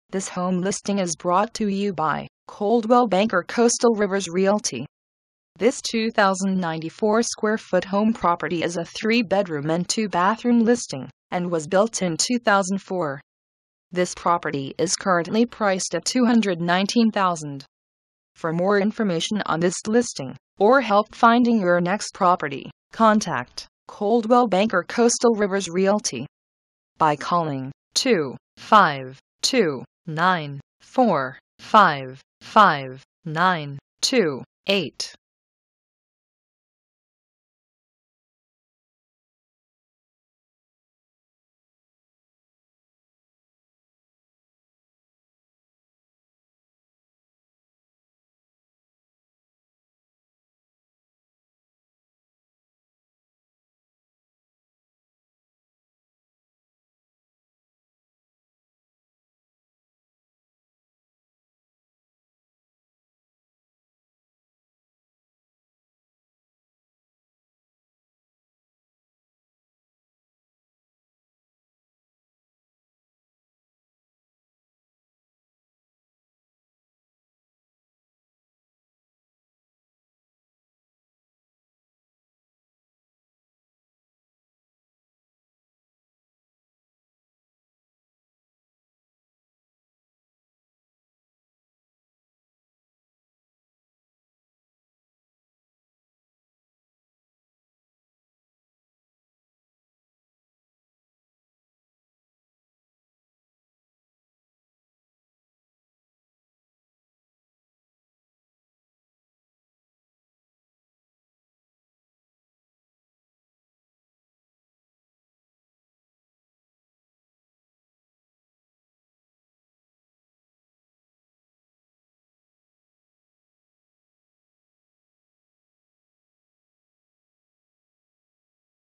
Washington Real Estate Home for Sale. $219,000 3bd/2ba. - Mary Ellen Adams of CoastalRivers.com
Listing: 31744
Property Type: Residential
Price: $219,000
Beds: 3
Baths: 2
Sq. Ft.: 2094
Year Built: 2004
Lot Size: 54RFx128x140x121
Number of Acres: 0.25
Units: 0
City: Washington
Address: 212 INDA WAY

Description: Easy living in this 3BR/2BA home in quiet cul-de-sac near hospital, shopping, etc. LR with vaulted ceiling, formal DR w/ hardwood floor, large kitchen w/ breakfast room/den, finished bonus room. Recent updates: new carpet, tile, SS appliances, induction oven, new counters, instant hot water, recessed lighting, security system, built-ins & landscaping. Private back yard w/ large patio & shed.

Listing Agent
Mary Ellen Adams

Broker
Coldwell Banker Coastal Rivers Realty
We never stop moving.